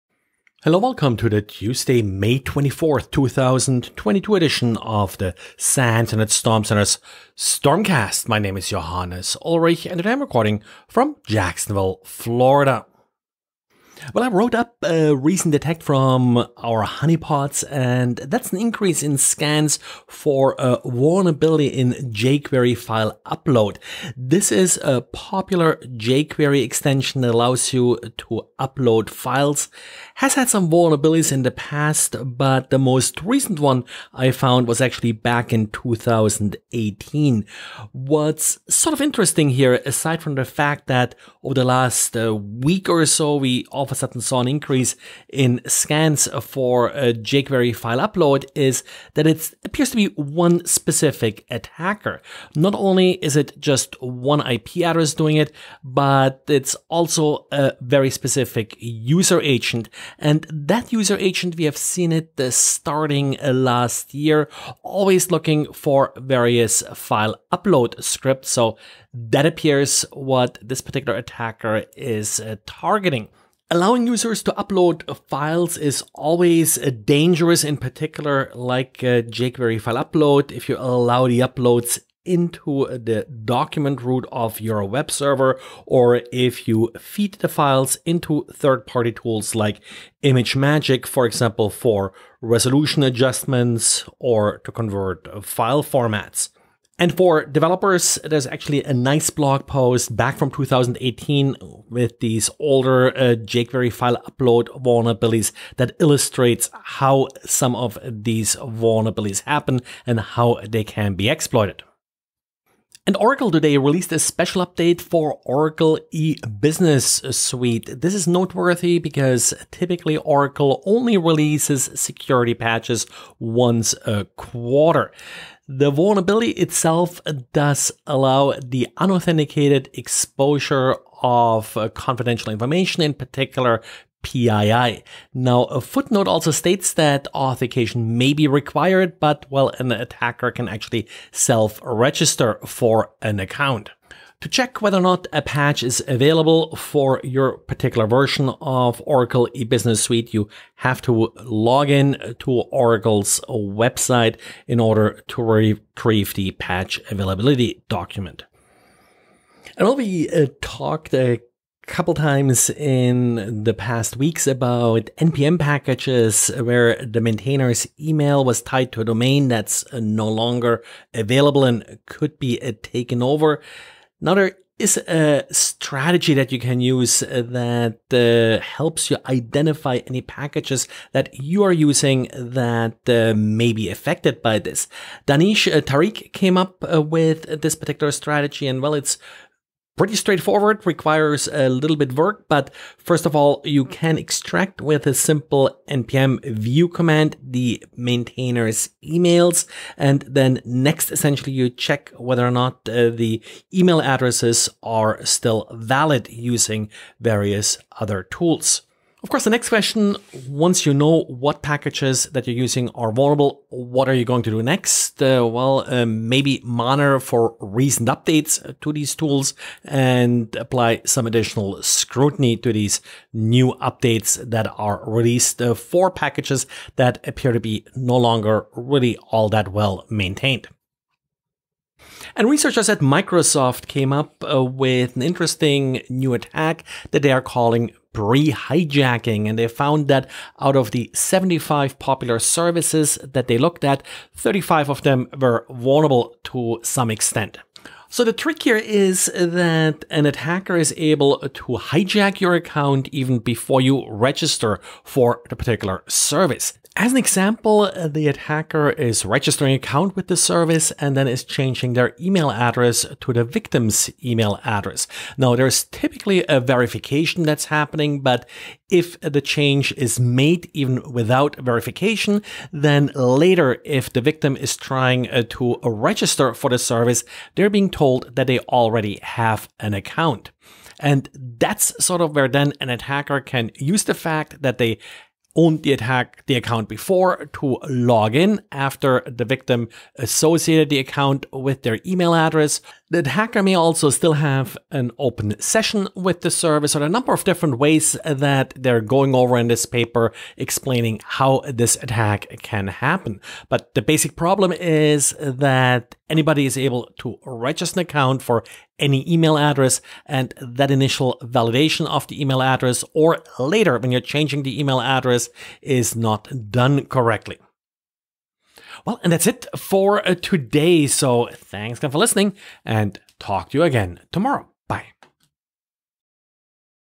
Network Security News Summary for Tuesday May 24th, 2022
jQuery-File-Upload Scans; Oracle OOB Patch; NPM Hijack Detection; Account Pre-Hijacking

How to find NPM dependencies vulnerable to account hijacking